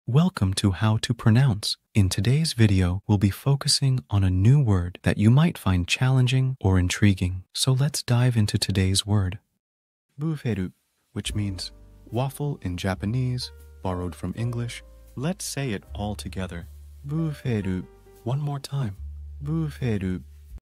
How to pronounce Vūferu
🌍 Learn a new language now Master the pronunciation of 'Vūferu' ヴーフェル 📖 - which means : Waffle in Japanese (borrowed from English) 📝 - with this quick and clear tutorial by How To Pronounce!

In this video 📽️, we'll guide you through the correct pronunciation of 'Vūferu' in Japanese, using native speaker examples 🗣️👂 and easy-to-follow phonetic breakdowns 🎵. Whether you're learning Japanese for travel ✈️, business 💼, or just for fun 🎉, our guide will help you perfect your pronunciation in no time ⏰.